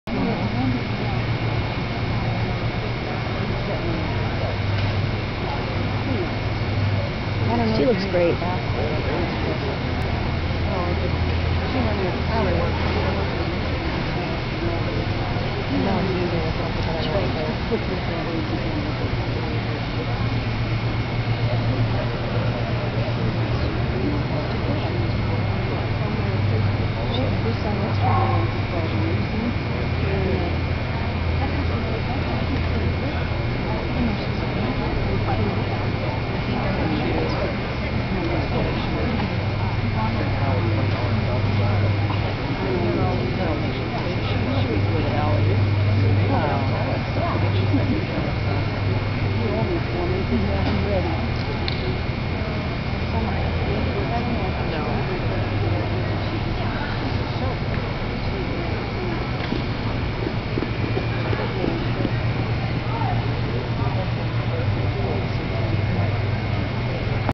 Capricorn's Classic Commotion
Puppy 12-18 mos bitch class  presentation in the Old English Sheepdog Club of America 2006 Specialty.